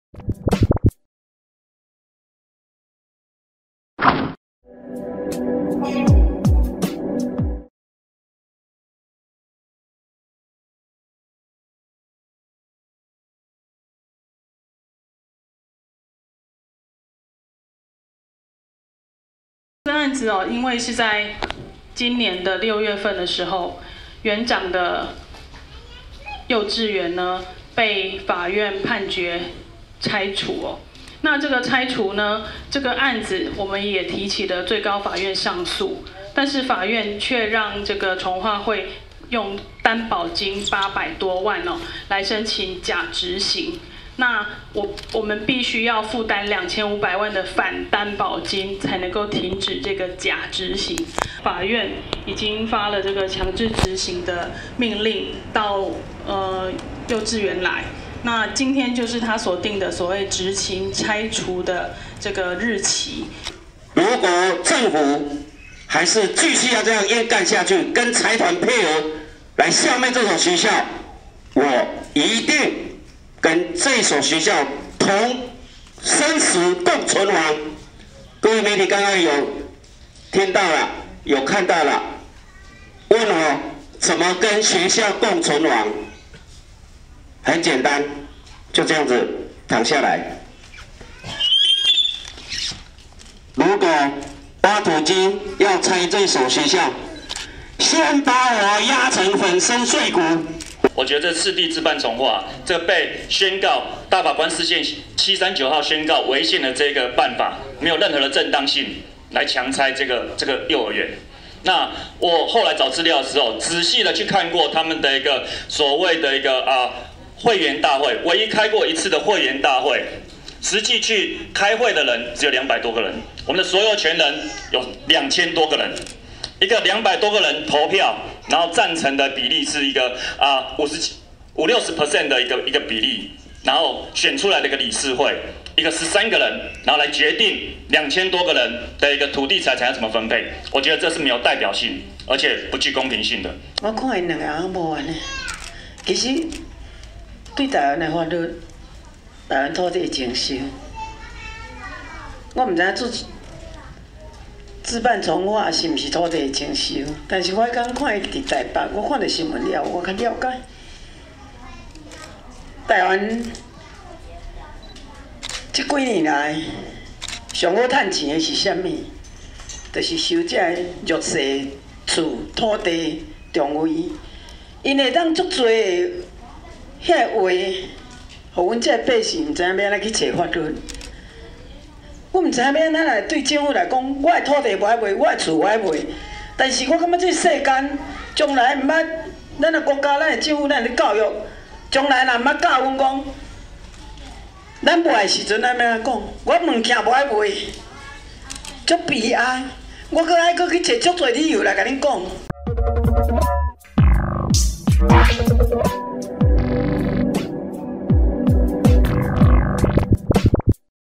20161121黎明幼兒園強拆暫緩 週三召開四方協調會決定命運
台中黎明幼兒園因拒絕參與自辦重劃，與重劃會纏訟多年，原定11月21日由法院行政執行單位到場現勘，但遭聲援團體包圍嗆聲，決定改日再測量。下午4點民眾轉至市府陳情，要求原地保留、重啟調處並停止強拆幼兒園，台中市地政局長張治祥回應，本週三(11/23)下午4點將到幼兒園召開四方協調會，邀集幼兒園方、重劃會、公民團體與地政局共同協調。